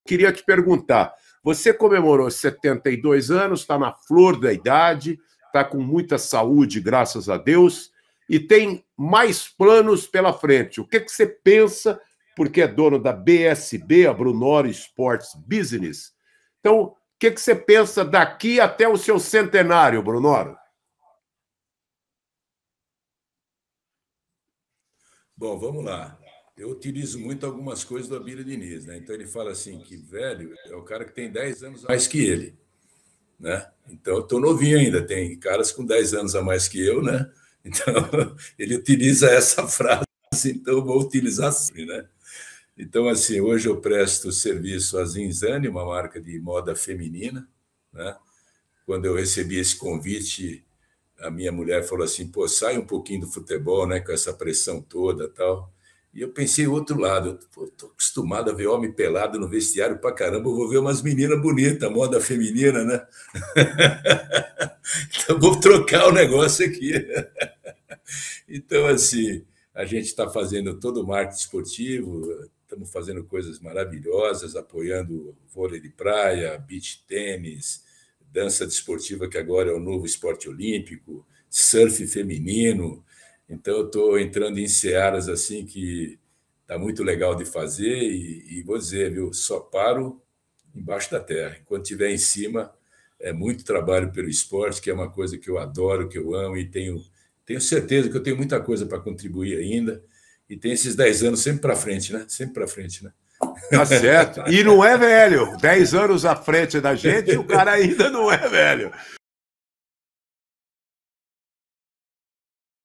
José Carlos Brunoro - Eu só paro embaixo da terra, até lá tenho muito para contribuir
Brunoro fala sobre os seus planos para o futuro.